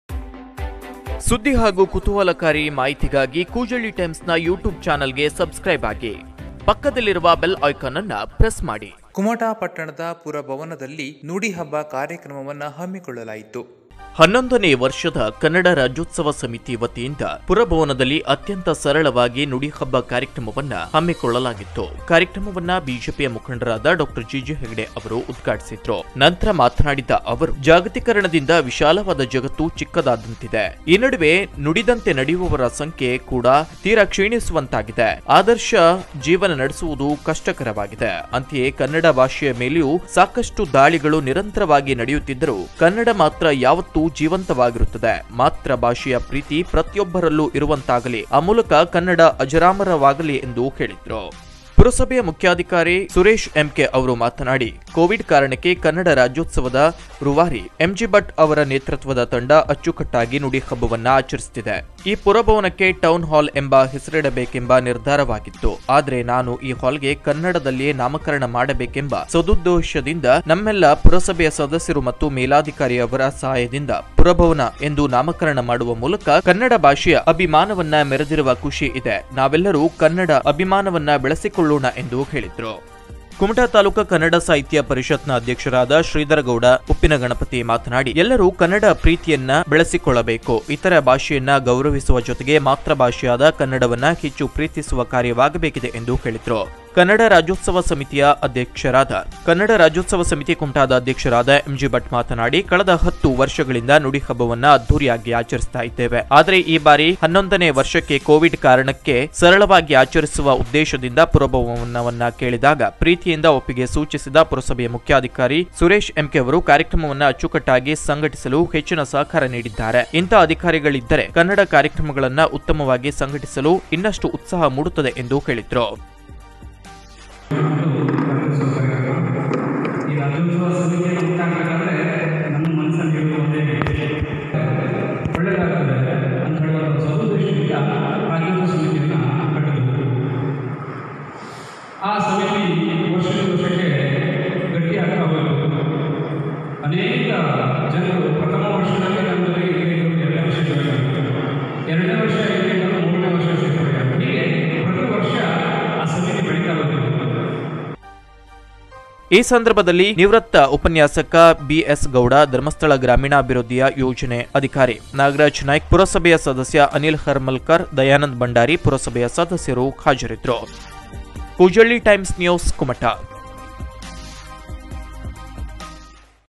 koojalli times TV
ಪುರಭವನದಲ್ಲಿ ಸರಳವಾಗಿ ನಡೆದ ನುಡಿ ಹಬ್ಬ ಕಾರ್ಯಕ್ರಮ
ರಾಜ್ಯೋತ್ಸವ ಸಮಿತಿ ಅಧ್ಯಕ್ಷರು ಎಂ.ಜಿ.ಭಟ್ ನೇತೃತ್ವದಲ್ಲಿ ಆಯೋಜನೆ
ಕೋವಿಡ್ ಕಾರಣಕ್ಕೆ  ಸರಳವಾಗಿ ಆಚರಿಸಿದ ನುಡಿಹಬ್ಬ